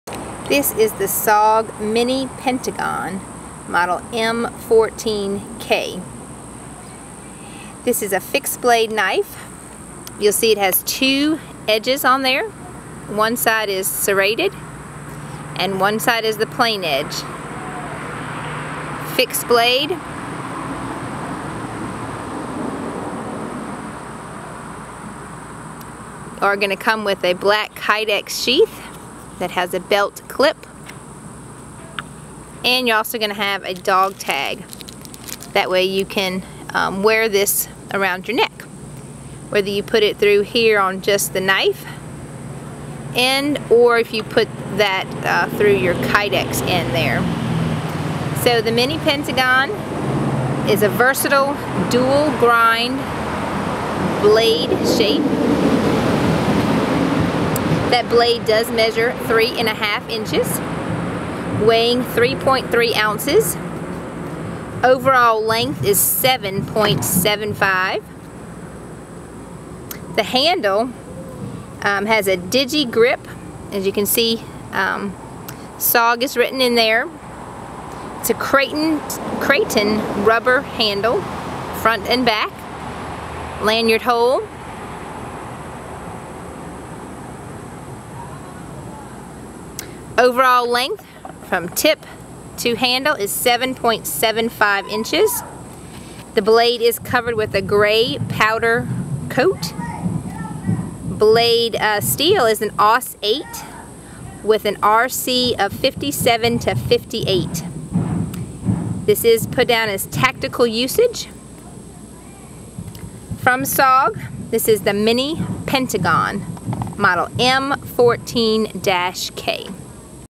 SOG Mini Pentagon Knife M14K-CP -video demo
As little brother to the SOG Pentagon, the SOG Mini Pentagon model M14 fits nicely concealed in your boot or vest. One edge of the AUS 8 blade is serrated while the other is finde edged. Thumb notches on both sides of the blade allow you to apply good downward pressure while gripping the textured Kraton handle. The Mini Pentagon comes with a hard plastic sheath for quick deployment and a chain.